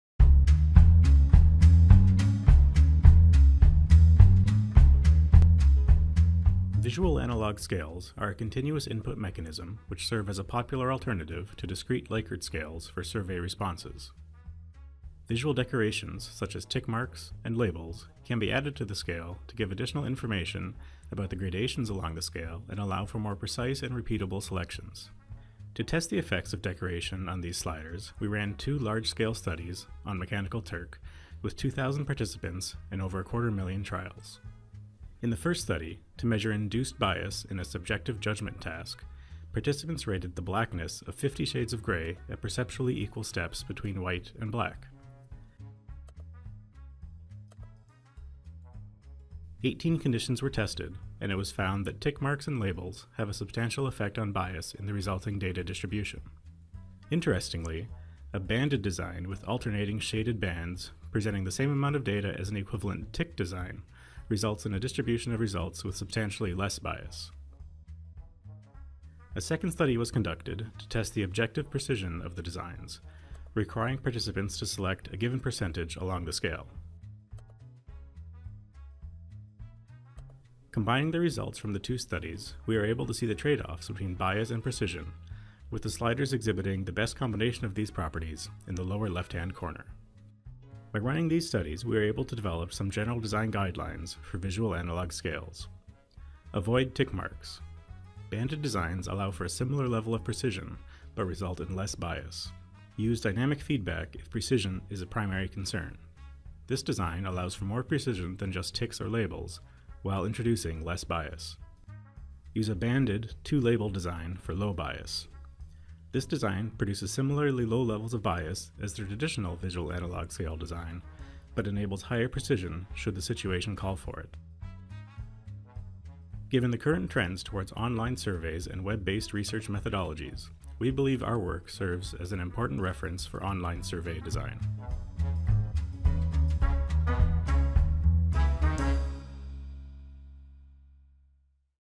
The Effect of Visual Appearance on the Performance of Continuous Sliders and Visual Analogue Scales
Sliders and Visual Analogue Scales (VASs) are input mechanisms which allow users to specify a value within a predefined range. At a minimum, sliders and VASs typically consist of a line with the extreme values labeled. Additional decorations such as labels and tick marks can be added to give information about the gradations along the scale and allow for more precise and repeatable selections. There is a rich history of research about the effect of labelling in discrete scales (i.e., Likert scales), however the effect of decorations on continuous scales has not been rigorously explored. In this paper we perform a 2,000 user, 250,000 trial online experiment to study the effects of slider appearance, and find that decorations along the slider considerably bias the distribution of responses received. Using two separate experimental tasks, the trade-offs between bias, accuracy, and speed-of-use are explored and design recommendations for optimal slider implementations are proposed.

Justin Matejka, Michael Glueck, Tovi Grossman & George Fitzmaurice. (2016).
CHI 2016 Conference Proceedings:
ACM Conference on Human Factors in Computing Systems.